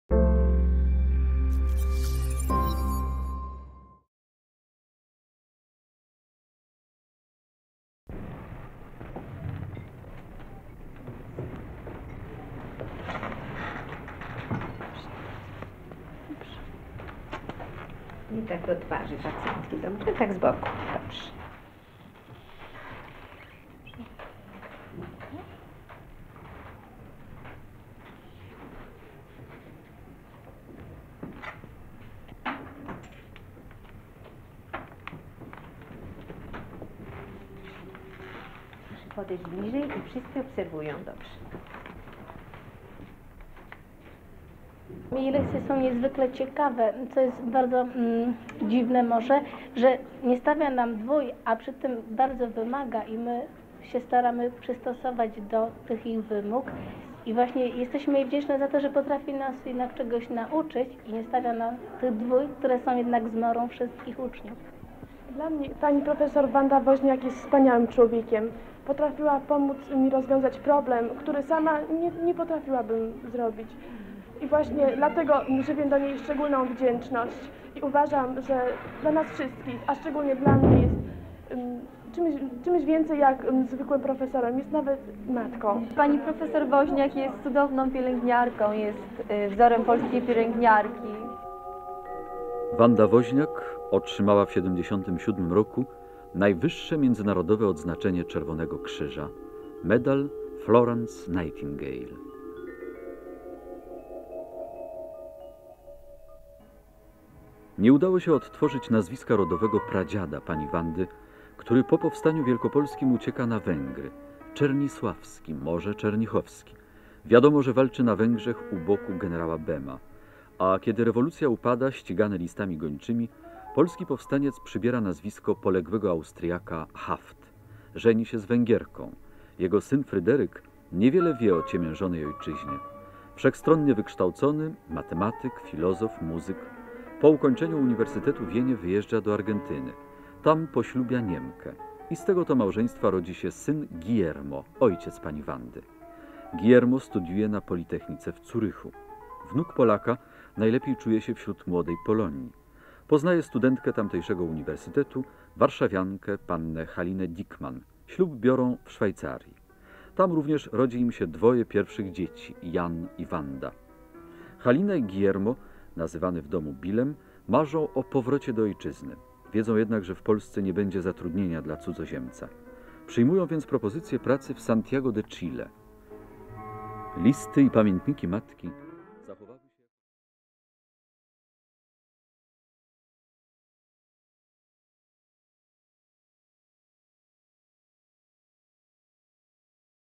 Ludzie i ich historie - "Pseudonim Iskra"
Film z cyklu „Życiorysy”, w którym prezentowane są sylwetki niezwykłych ludzi. Spotkanie z laureatką międzynarodowego medalu, imienia Florence Nightingale − Wandą Woźniak, pseudonim powstańczy „Iskra”. W żyłach pani Wandy płynie krew polska, węgierska i niemiecka. Jej rodzina mieszkała w Santiago de Chile. W 1933 roku, po śmierci ojca, matka wraz z Wanda i jej bratem, przenieśli się do Warszawy. W 1940 roku ukończyła Warszawską Szkołę Pielęgniarstwa. Działała w konspiracji od października 1939 roku.